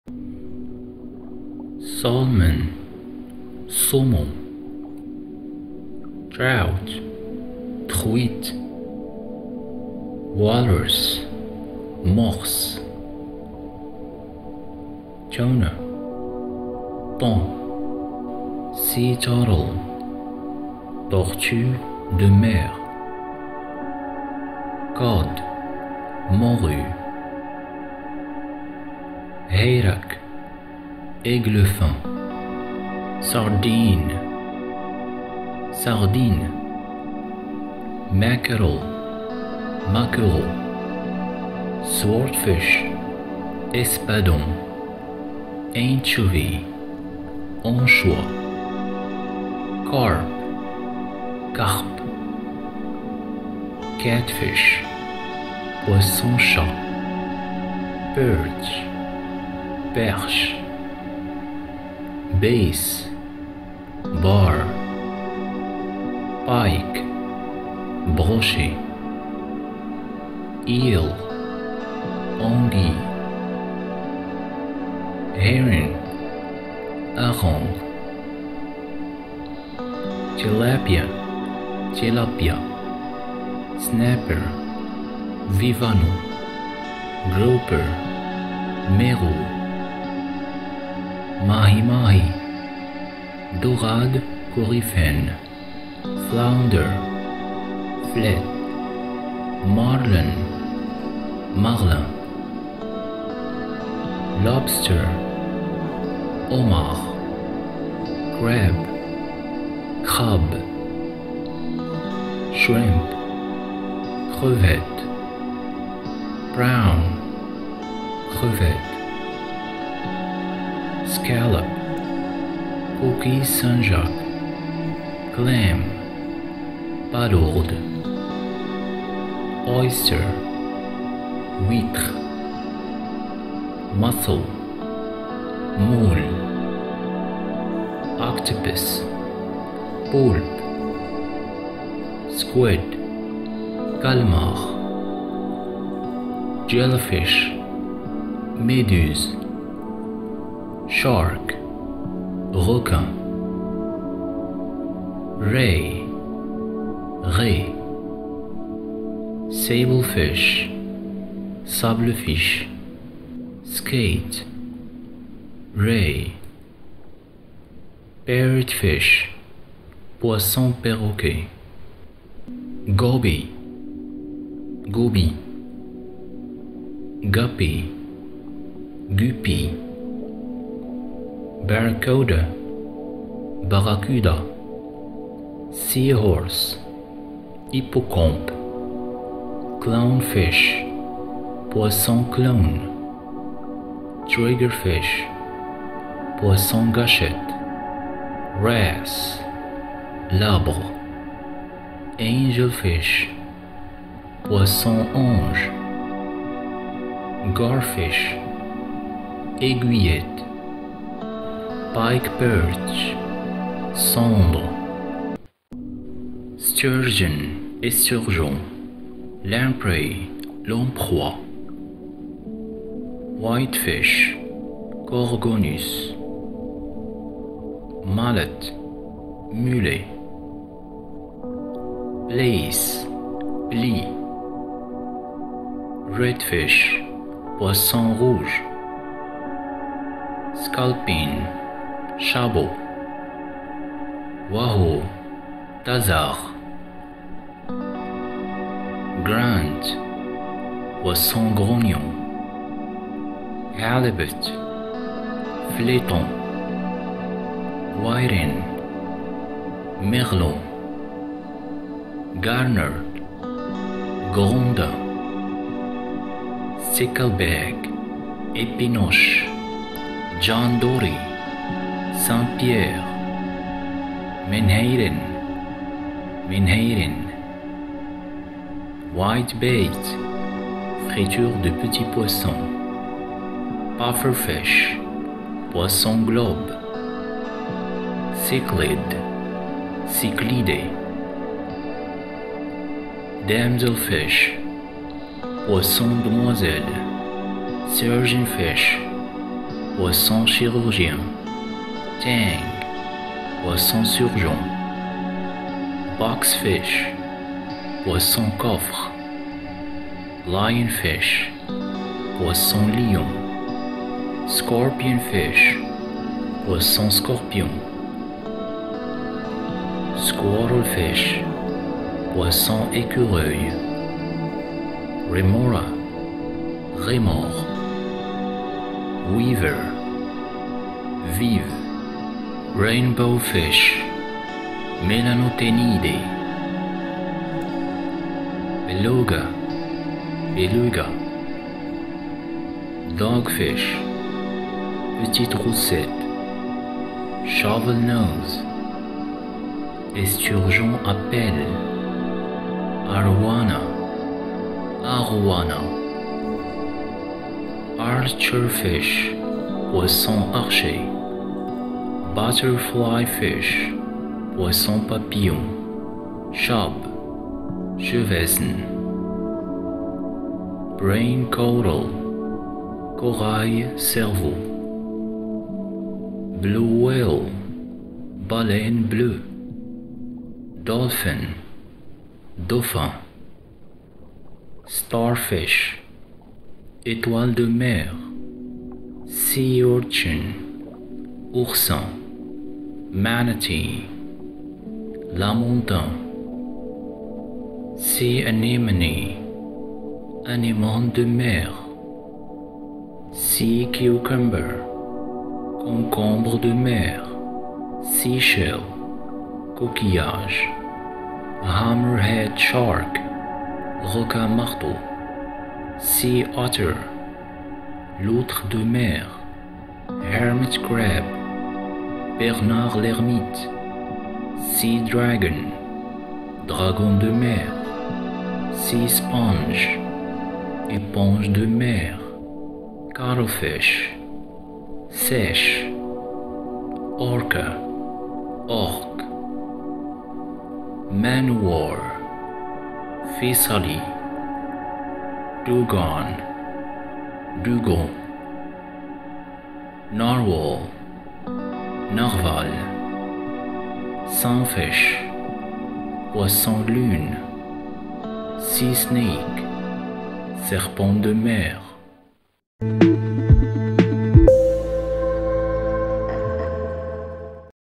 Voyage through Vocabulary: Sea Creatures in English and French
Dive into the enchanting world beneath the waves with our captivating vocabulary video on sea creatures!

Explore the depths of the ocean as we present stunning images of each marine marvel, accompanied by clear pronunciations in both English and French.

Whether you're looking to expand your linguistic skills or simply want to discover the incredible inhabitants of the sea, "Apprendre c'est Vivre" is your gateway to oceanic knowledge and language proficiency.